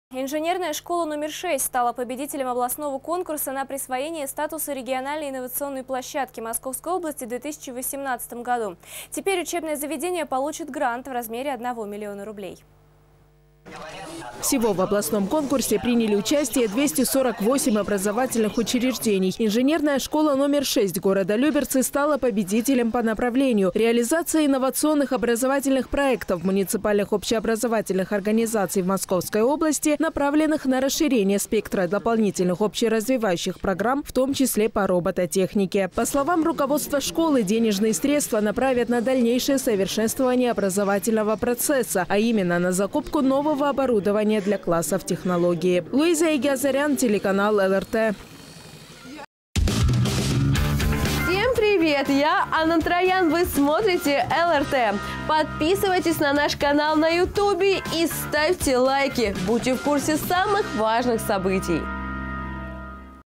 Инженерная школа получит грант на 1 миллион рублей
Инженерная школа No6 стала победителем областного конкурса на присвоение статуса Региональной инновационной площадки Московской области в 2018 году. Теперь учебное заведение получит грант в размере 1 миллиона рублей.

Всего в областном конкурсе приняли участие 248 образовательных учреждений. Инженерная школа No6 города Люберцы  стала победителем по направлению: «Реализация инновационных образовательных проектов муниципальных общеобразовательных организаций в Московской области, направленных на расширение спектра дополнительных общеразвивающих программ для детей, в том числе по робототехнике».

По словам руководства школы, денежные средства направят на дальнейшее совершенствование образовательного процесса, а именно на закупку нового оборудования для классов технологии.

Луиза Егиазарян